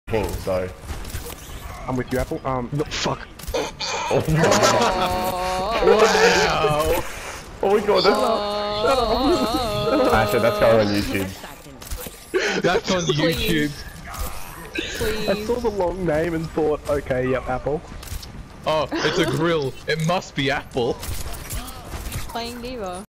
Alexander Mistaking Mercury for His Gril
Big Ash mistaking poor mercury for his GF during a comp match, how embarrassing. Also is Ashpple a good relationship name for them?